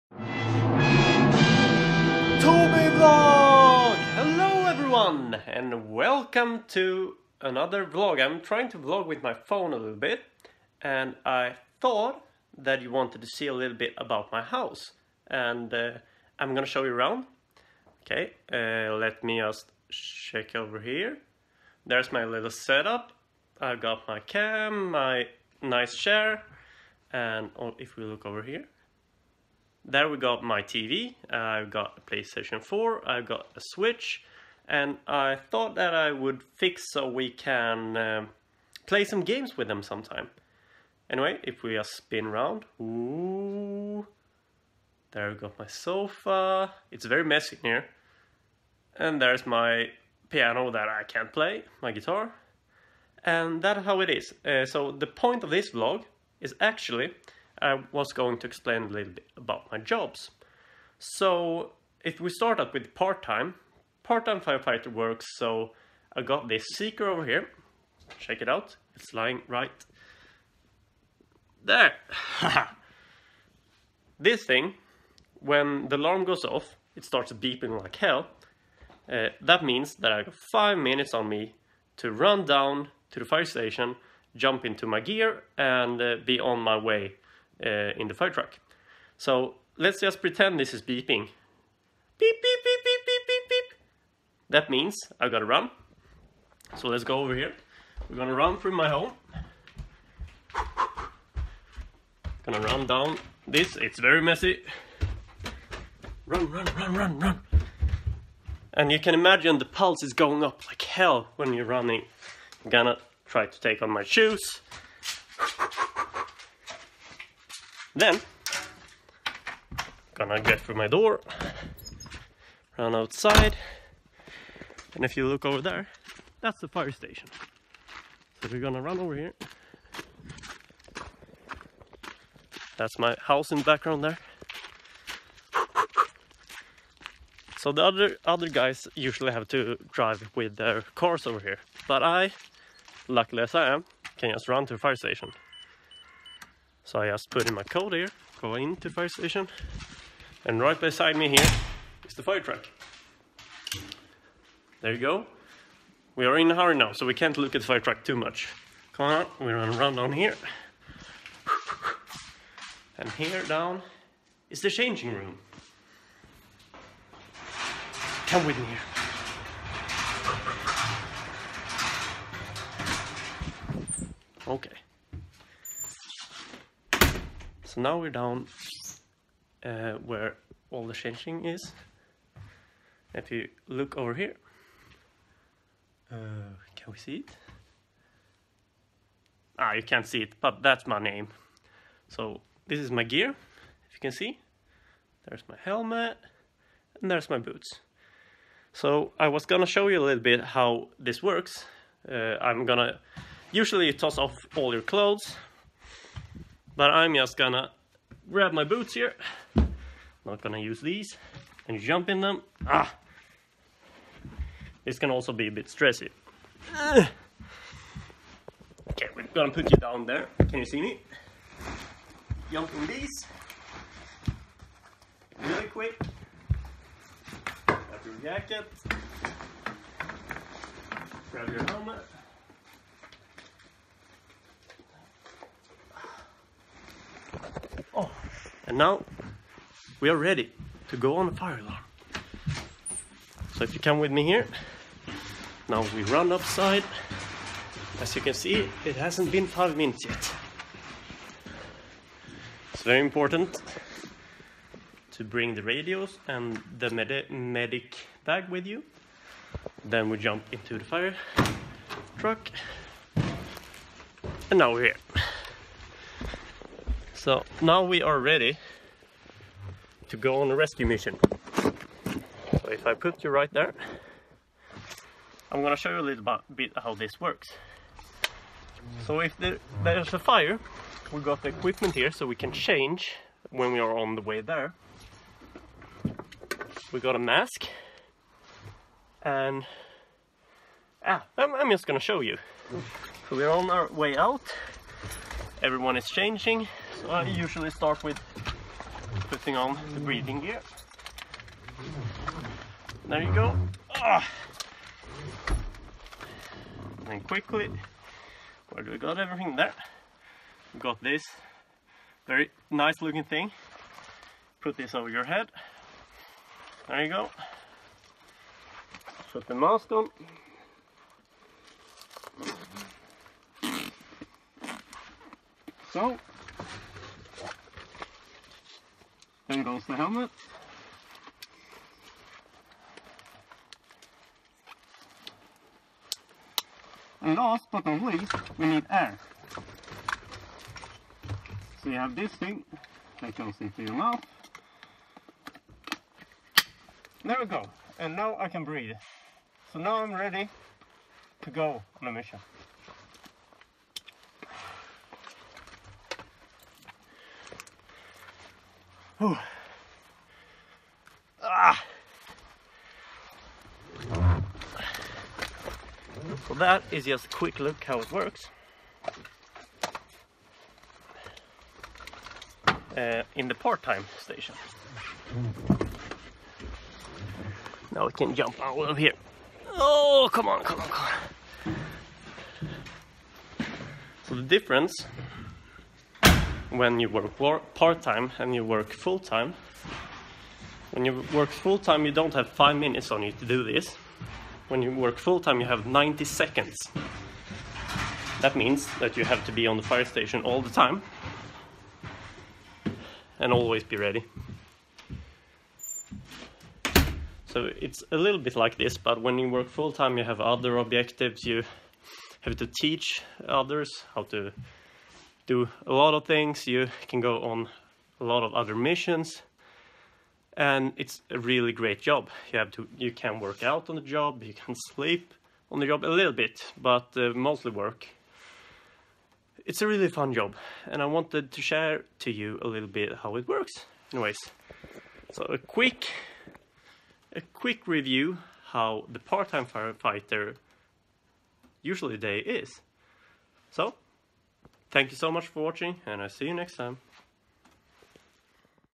Firefighter JOB-VLOG - Partime Alarm
Thought I might tell you a little bit about my 3 jobs as a firefighter!